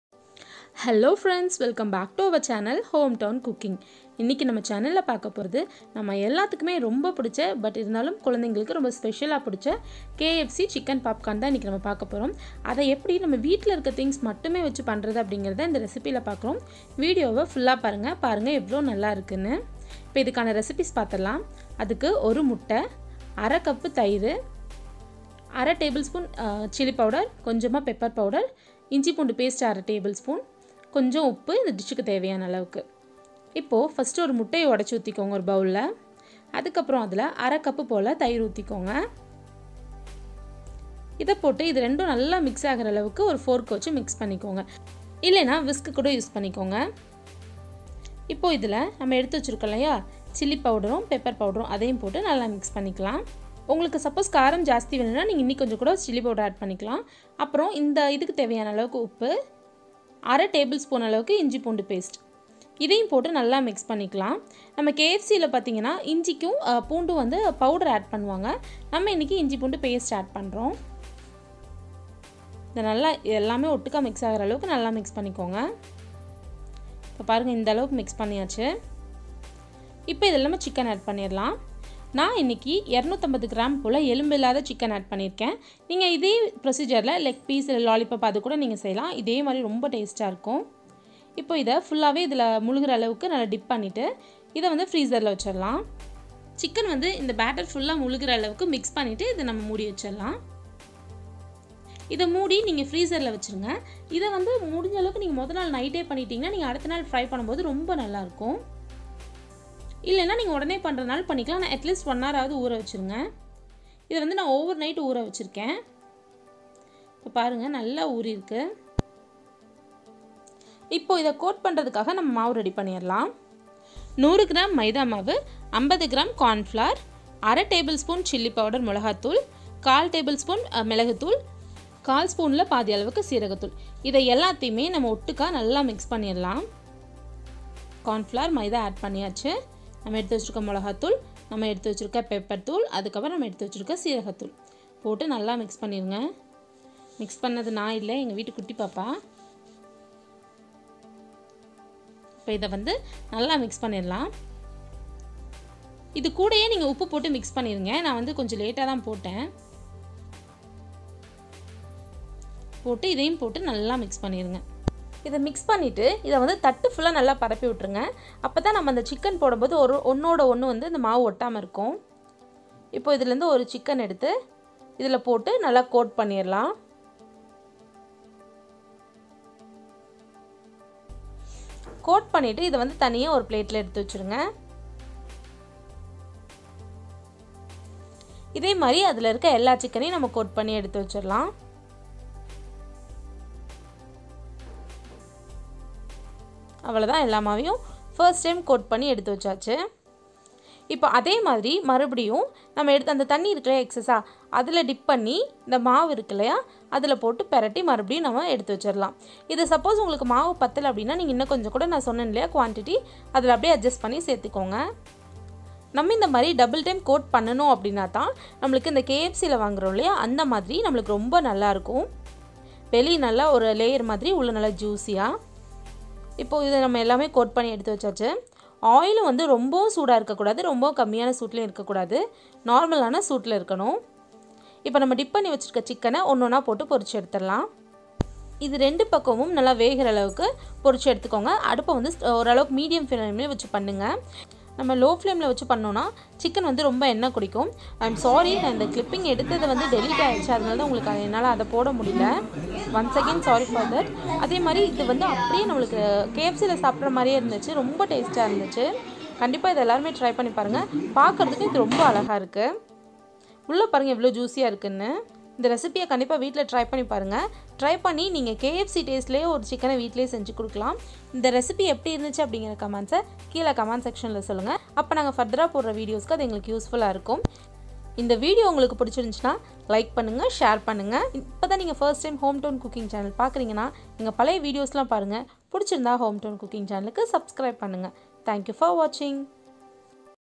100% Perfect KFC Chicken Popcorn / How to make KFC Chicken recipe in Tamil / HomeTown Cooking
Hai Friends

              Today we going to see how to make a KFC Style fried Chicken Popcorn recipe in Tamil.

PLEASE LEAVE YOUR VALUABLE COMMENTS IN COMMENTS SECTION FOR OUR FURTHER VIDEOS.